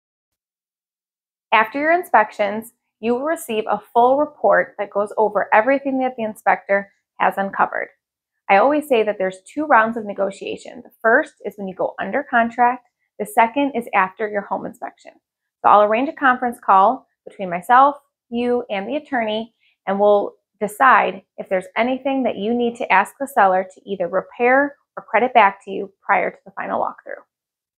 Inspection and Review | Buying a Home with The Glover Team | Top Producing Real Estate Agents
1100+ Transactions | $232+ Million in Sales
5 Full Time Agents | 4 Generations Experience
MORE 20 under 40 | Top 1% Sales

Realtor in Downers Grove and surrounding DuPage County